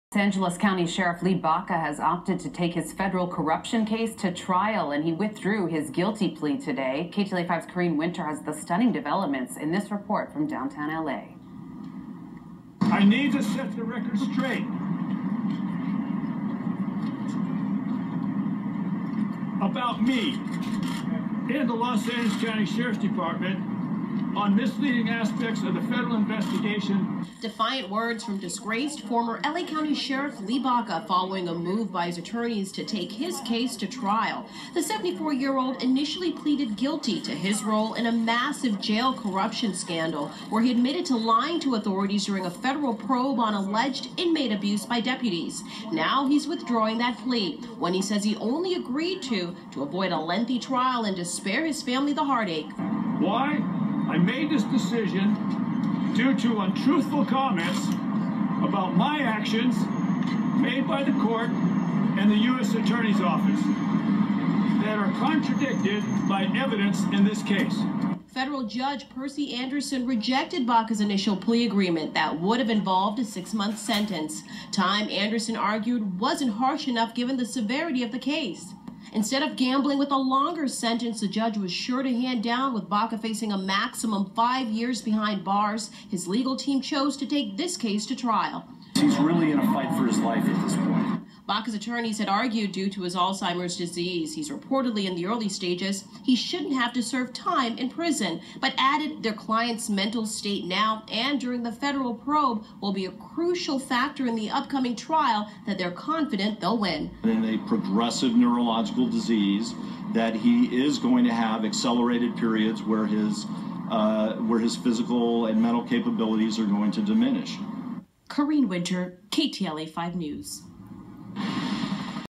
Lee Baca - Withdrawing Guilty Plea
Retired Sheriff of Los Angeles County withdraws guilty plea and will face Trial later on this year.